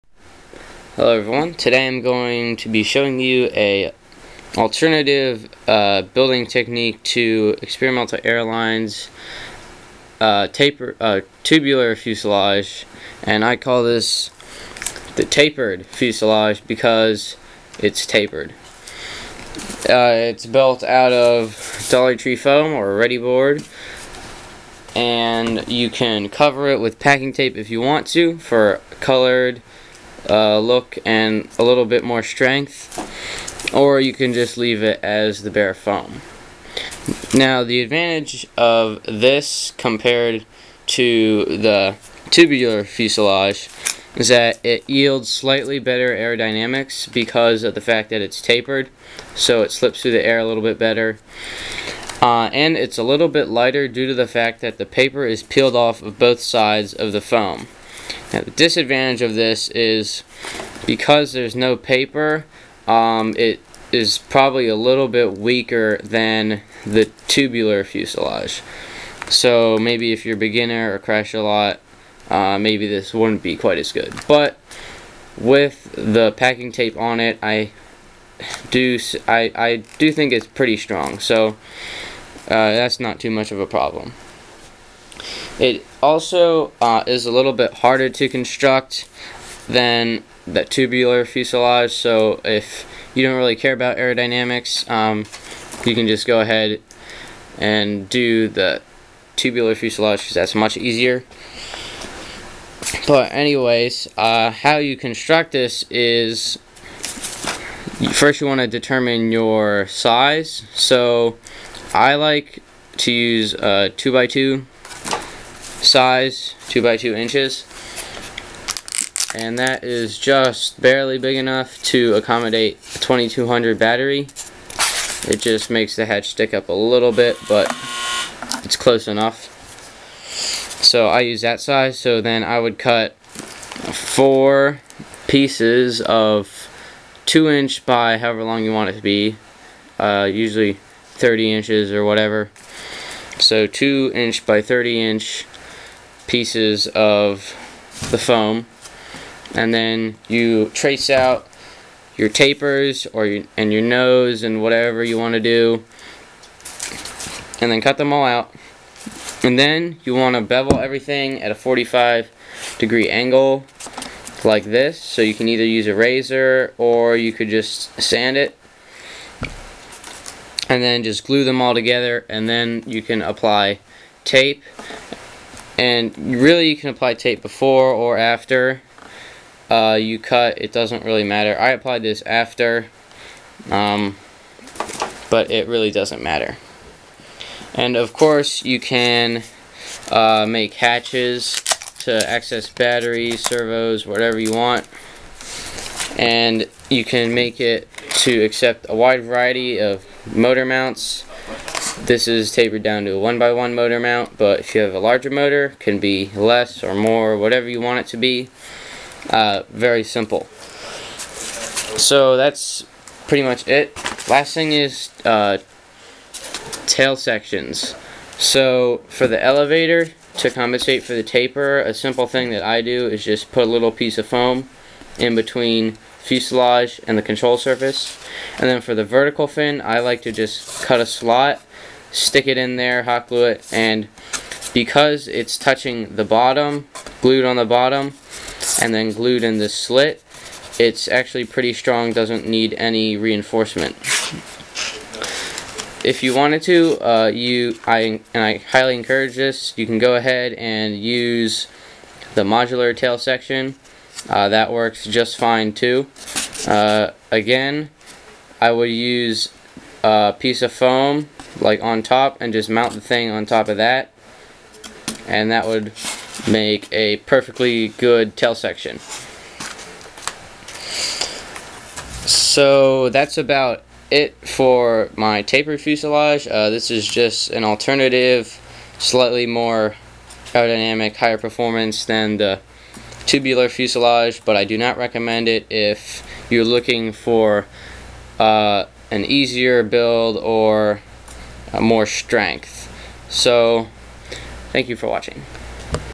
Foamboard Scratchbuilding Techniques: Tapered Fusulage
Made out of dollar tree foam. Designed to be used as an alternative to ExperimentalAirlines' tubular fuselage.